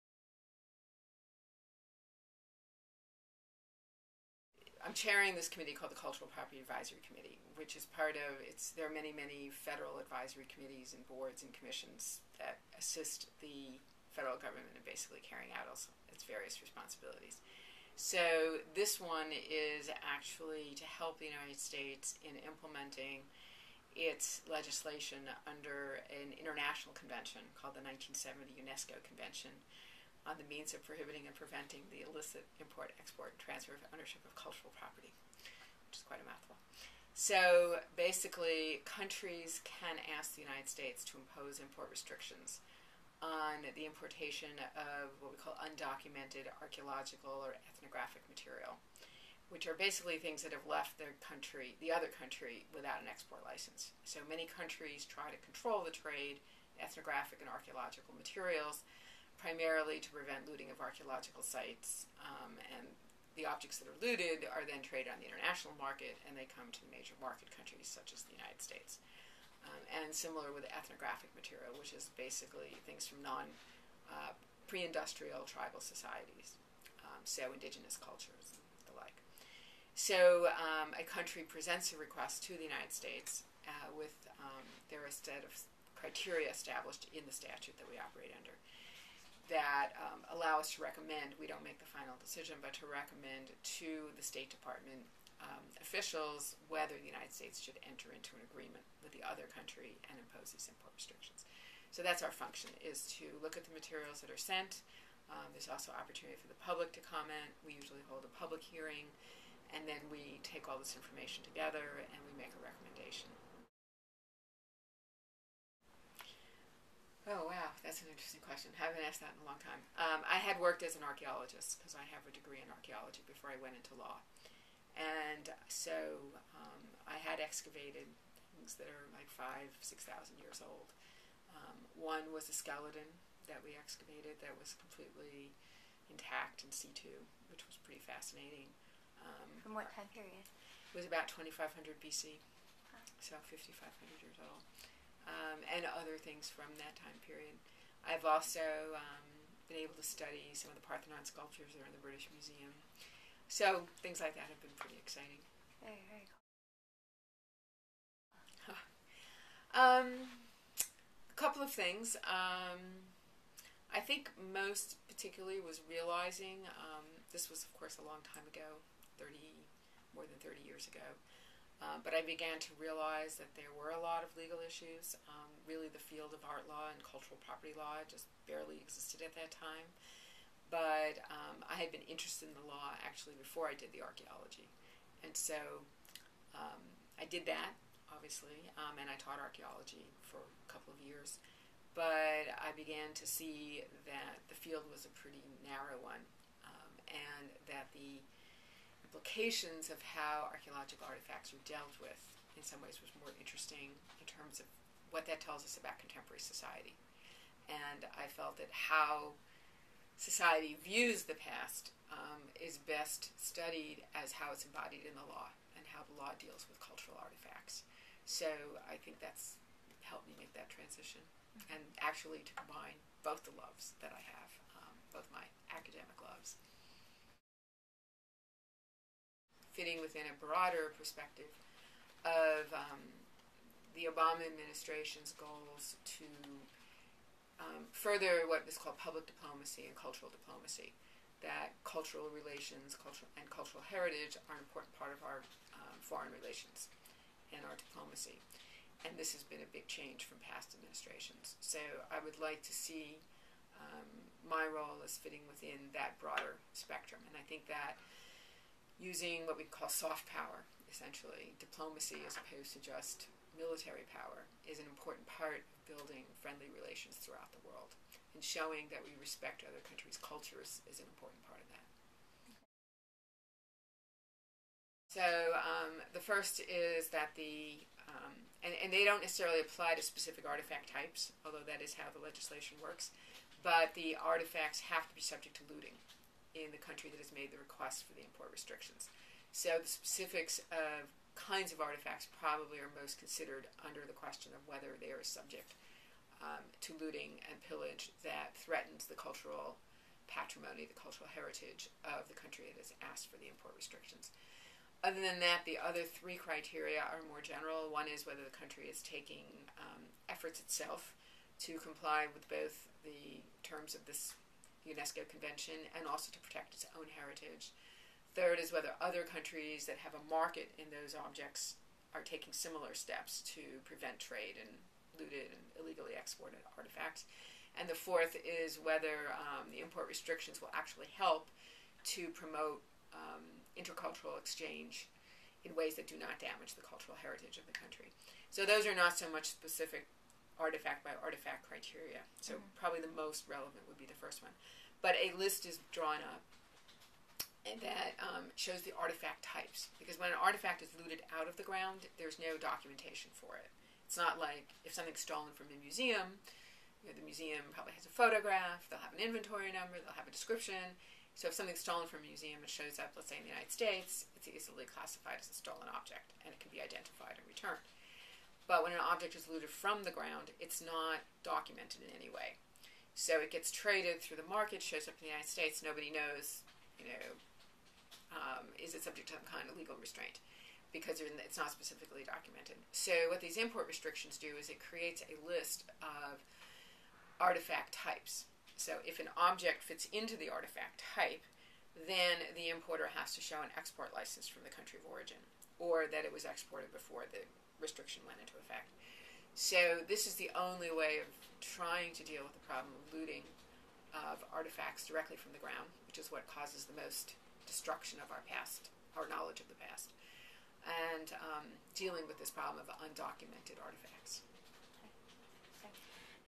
PATTY GERSTENBLITH
President Obama announced his intent to appoint DePaul University professor Patty Gerstenblith to a key Administration post on Friday. Gerstenblith, a Distinguished Research Professor and Director for the Center for Art, Museum and Cultural Heritage Law at DePaul's College of Law, was named as the Chairman of the Cultural Property Advisory Committee.